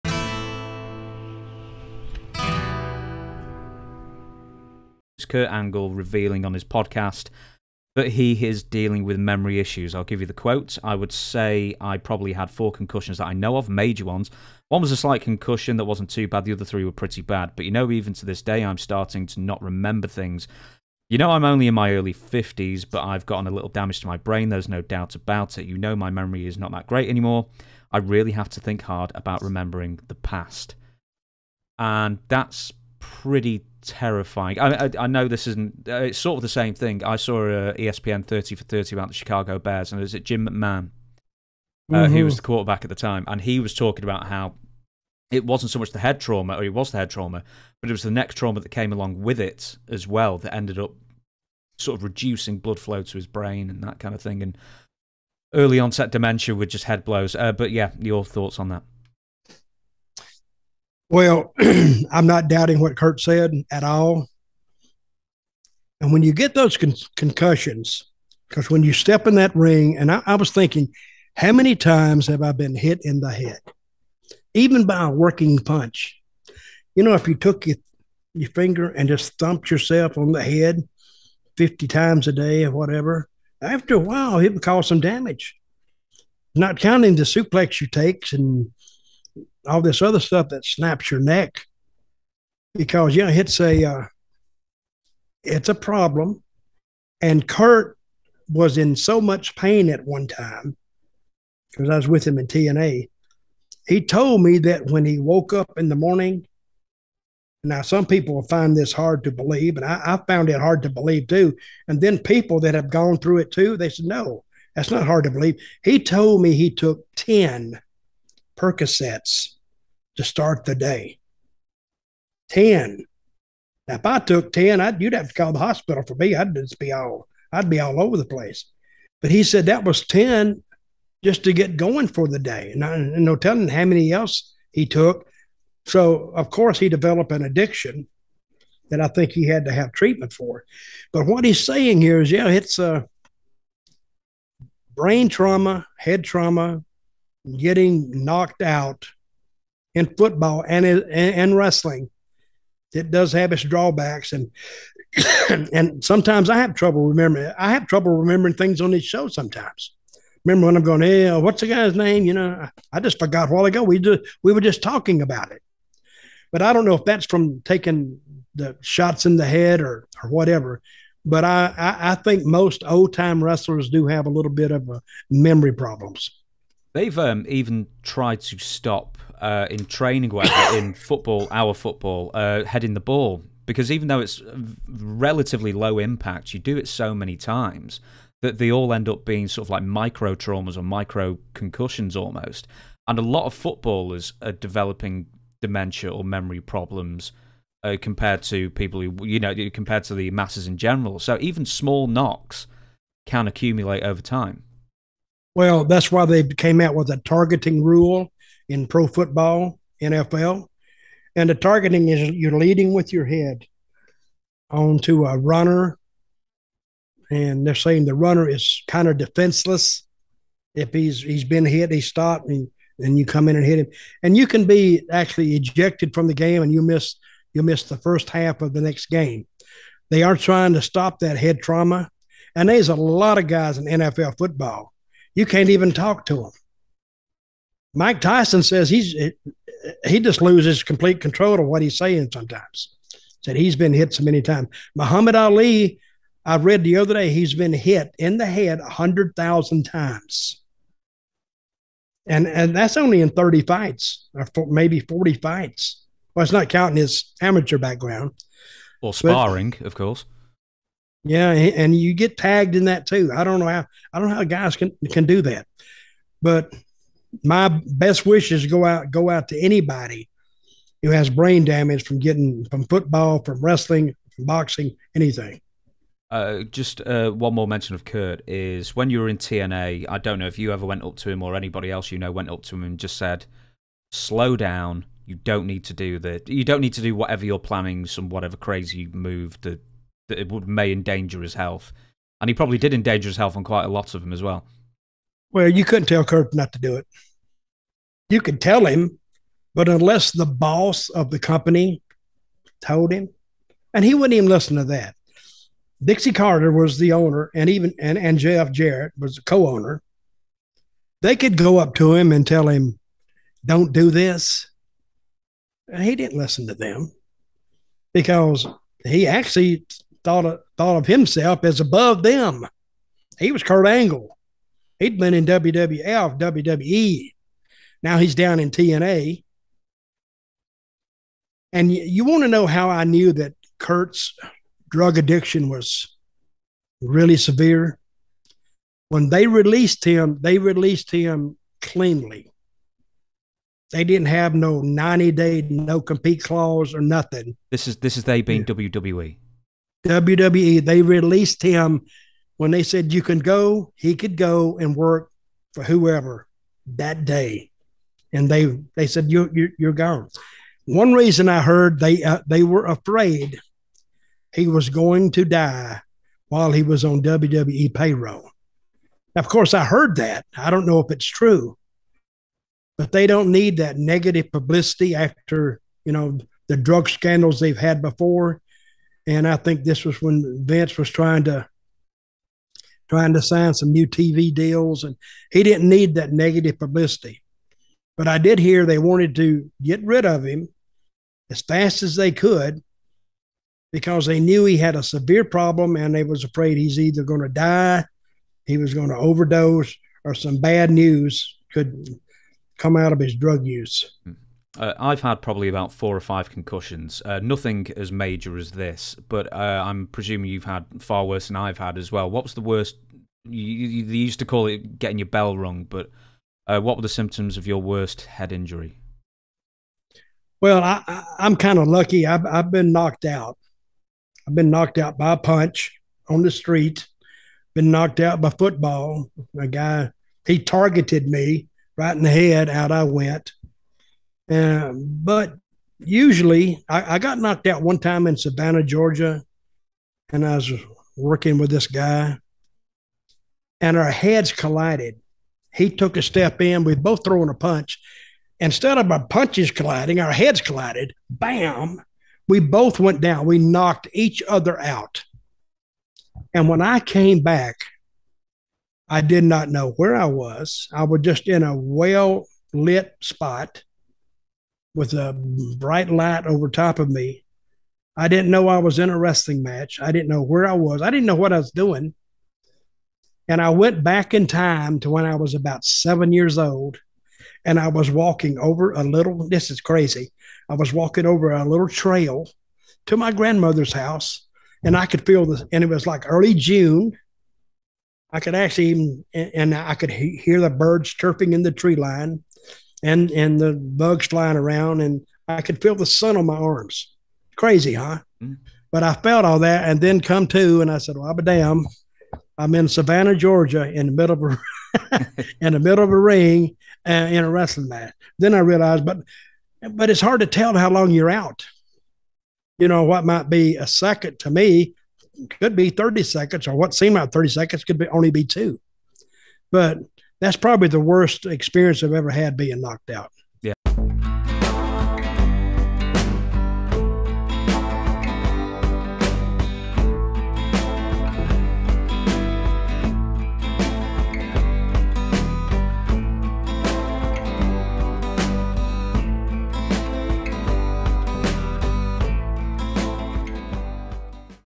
Dutch Mantell on Kurt Angle Dealing with Memory Loss, Concussions & Not Toning Down His In Ring Work
Dutch Mantell's Tales From a Dirt Road
Dutch Mantell's The World According to Dutch